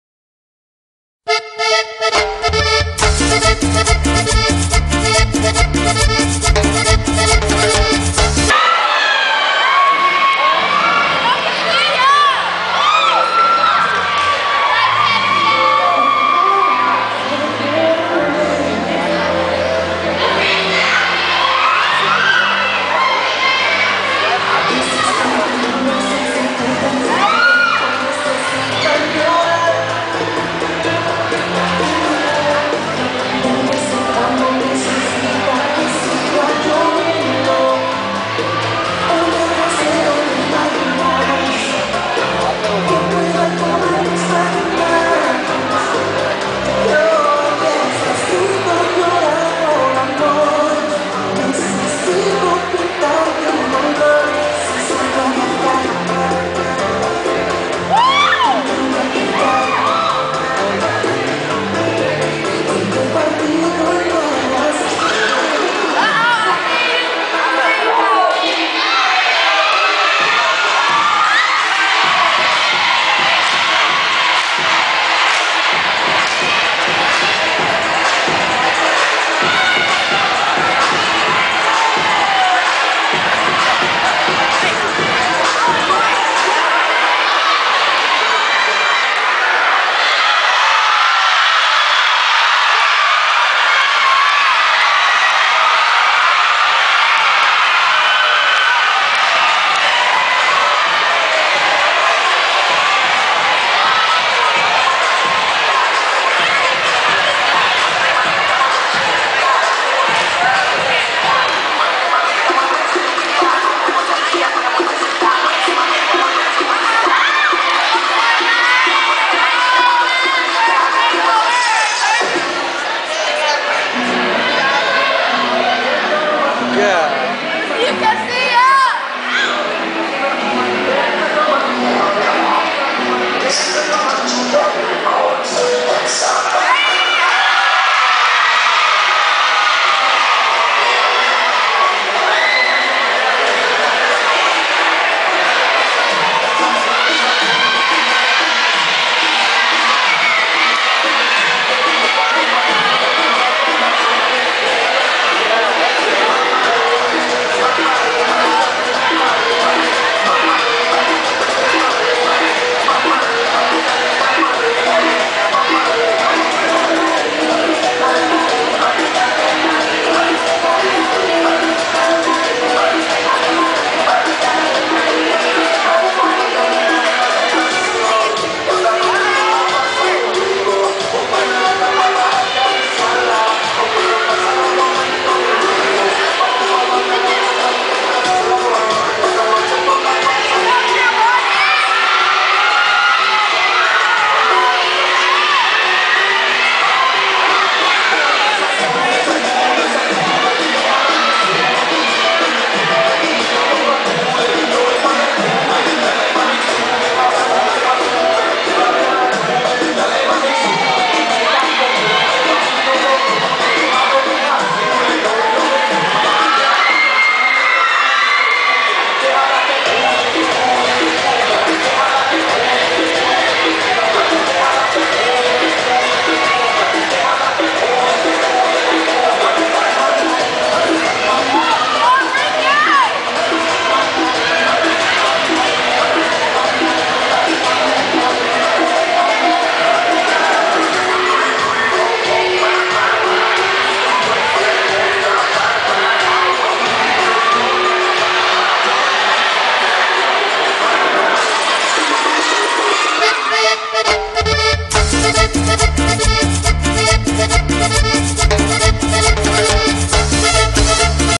DESOTO FRESHMAN CINCO DE MAYO 2009
FRESHMAN MEXICANS DANCING TO THREE DIFFERENT TYPES OF MUSIC